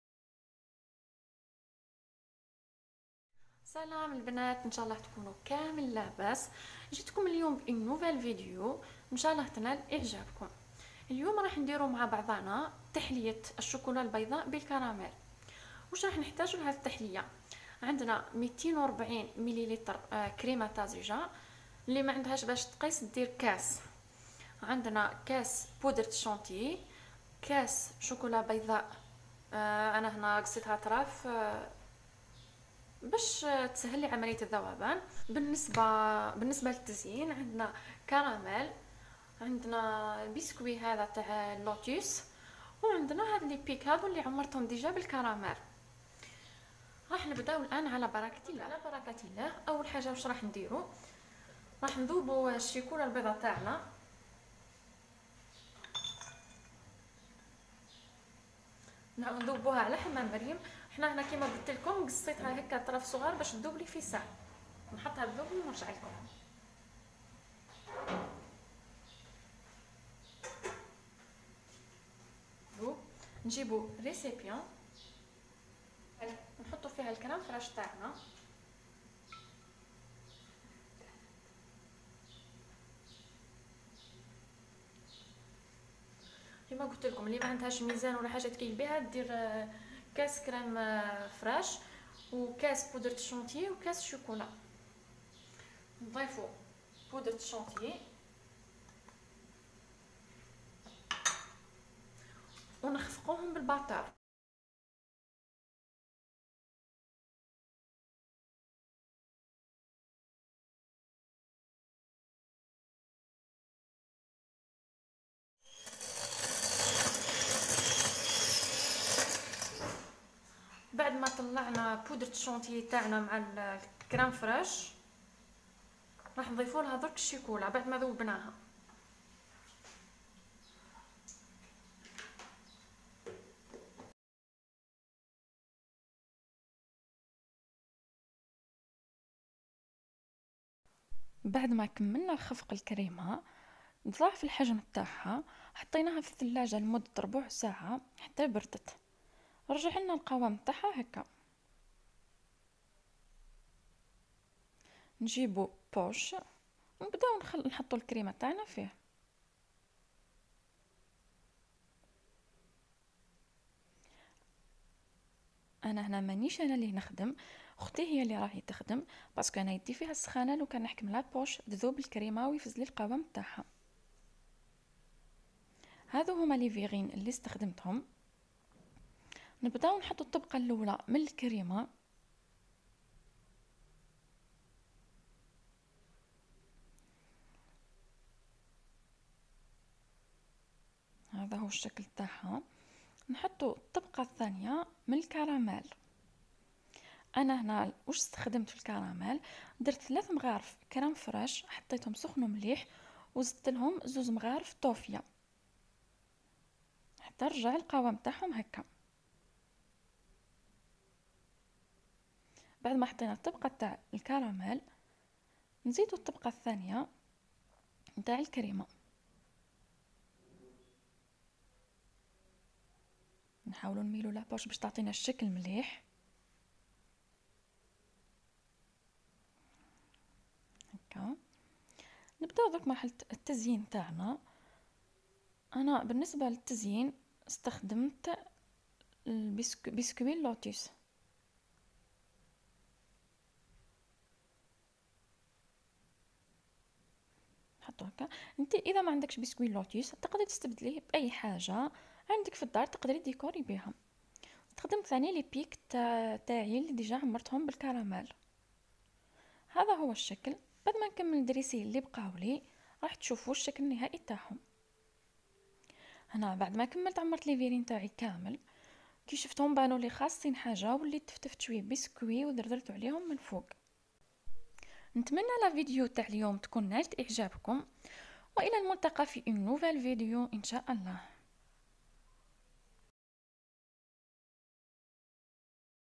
تحلية الشوكولاته البيضاء بالكراميل و بسكويت لوتيس Lotus 🍫🍫😋 بثلاث مكونات فقط ⁦⁩⁦❤️⁩ سهلة و سريعة
الوصفة :
كأس كريمة طازجة
كأس بودرة الشانتيي
كأس شوكولا بيضاء
للتزيين :

بسكويت لوتيس
كراميل